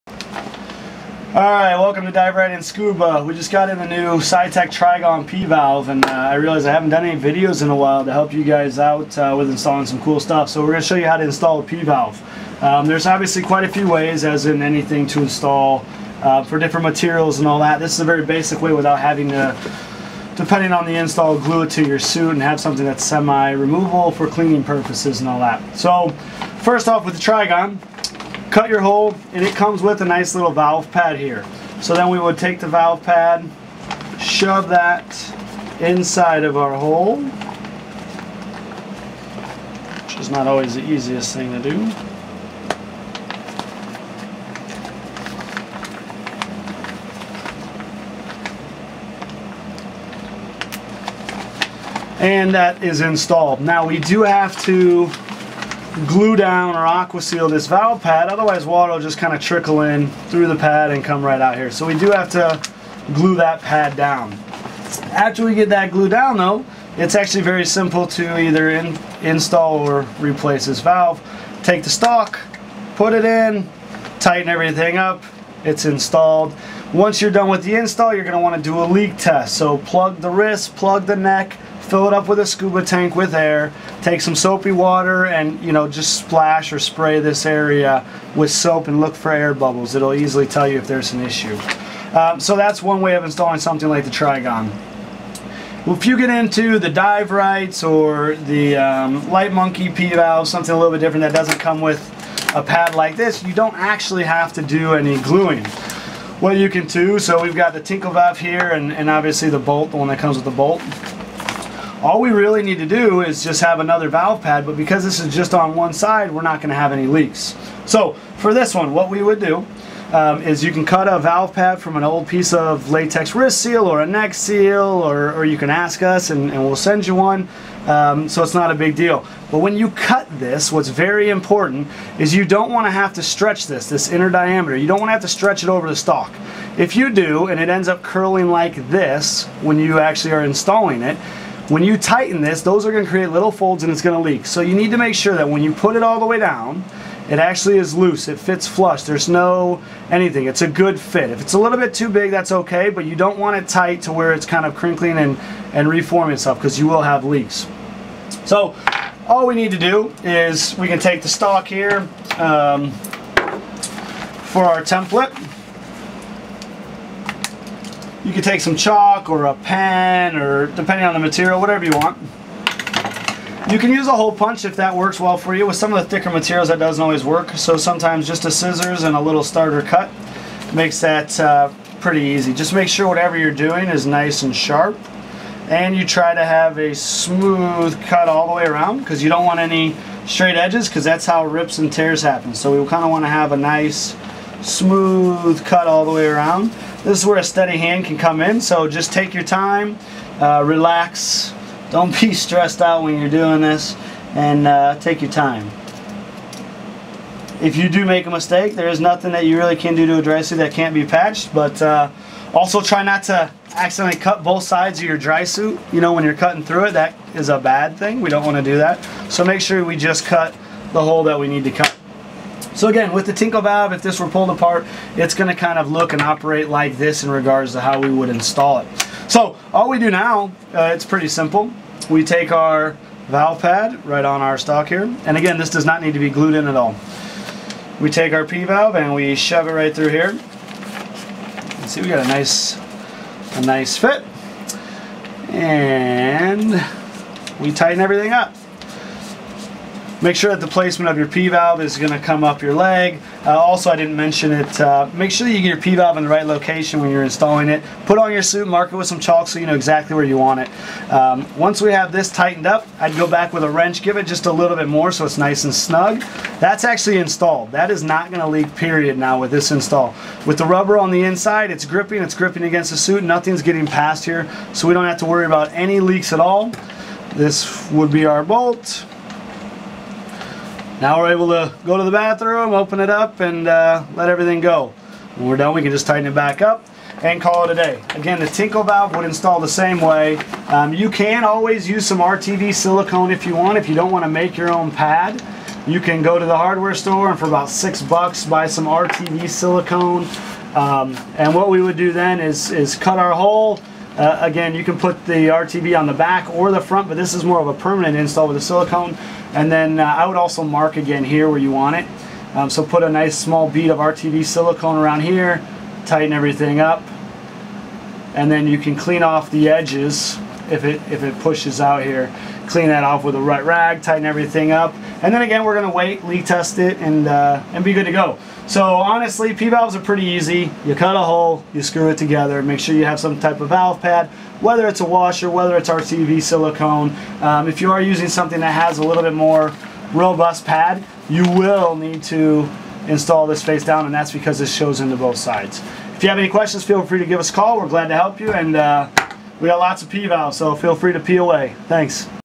Installing a P-valve into your drysuit
Dive Right In Scuba giving a quick rundown on installing a P-valve into your drysuit.  There are always multiple ways to install these, and even different materials.  Please call our experts if you have ANY questions at all!